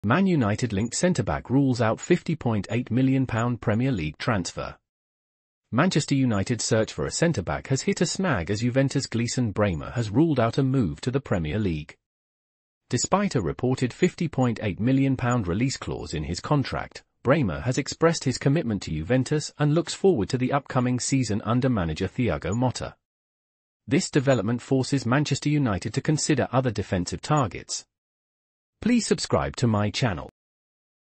Man United-linked centre-back rules out £50.8m Premier League transfer
Manchester United's search for a centre-back has hit a snag as Juventus' Gleison Bremer has ruled out a move to the Premier League. Despite a reported £50.8m release clause in his contract, Bremer has expressed his commitment to Juventus and looks forward to the upcoming season under manager Thiago Motta. This development forces Manchester United to consider other defensive targets.